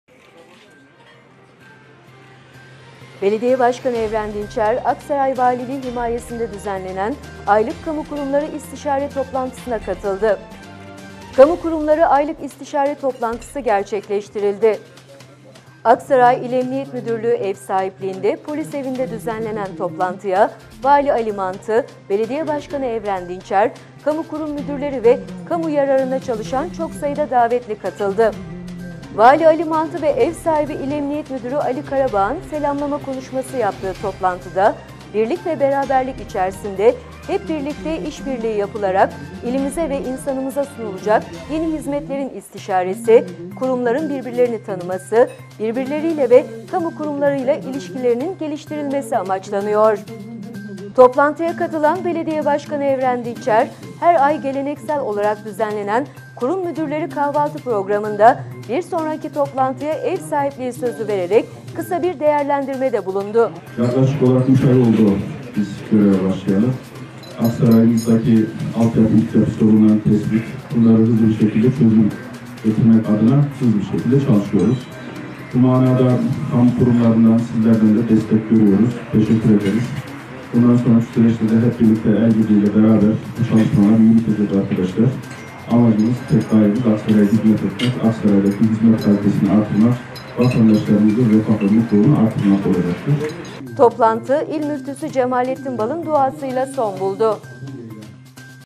KAMU KURUMLARI AYLIK İSTİŞARE TOPLANTISI GERÇEKLEŞTİRİLDİ
BELEDİYE BAŞKANI EVREN DİNÇER, AKSARAY VALİLİĞİ HİMAYESİNDE DÜZENLENEN AYLIK KAMU KURUMLARI İSTİŞARE TOPLANTISINA KATILDI.
AKSARAY İL EMNİYET MÜDÜRLÜĞÜ EV SAHİPLİĞİNDE POLİS EVİ’NDE DÜZENLENEN TOLANTIYA VALİ ALİ MANTI, BELEDİYE BAŞKANI EVREN DİNÇER, KAMU KURUM MÜDÜRLERİ, KAMU YARARINA ÇALIŞAN ÇOK SAYIDA DAVETLİ KATILDI.
VALİ ALİ MANTI VE EV SAHİBİ İL EMNİYET MÜDÜRÜ ALİ KARABAĞ’IN SELAMLAMA KONUŞMASI YAPTIĞI TOPLANTIDA BİRLİK VE BERABERLİK İÇERİSİNDE HEP BİRLİKTE İŞ BİRLİĞİ YAPILARAK İLİMİZE VE İNSANIMIZA SUNULACAK YENİ HİZMETLERİN İSTİŞARESİ, KURUMLARIN BİRBİRLERİNİ TANIMASI, BİRBİRLERİYLE VE KAMU KURUMLARIYLA İLİŞKİLERİNİN GELİŞTİRİLMESİ AMAÇLANIYOR.
 TOPLANTIYA KATILAN BELEDİYE BAŞKANI EVREN DİNÇER, HER AY GELENEKSEL OLARAK DÜZENLENEN KURUM MÜDÜRLERİ KAHVALTI PROGRAMINDA BİR SONRAKİ TOPLANTIYA EV SAHİPLİĞİ SÖZÜ VEREREK KISA BİR DEĞERLENDİRMEDE BULUNDU.
TOPLANTI İL MÜFTÜSÜ CEMALLETTİN BAL’IN DUASIYLA SON BULDU.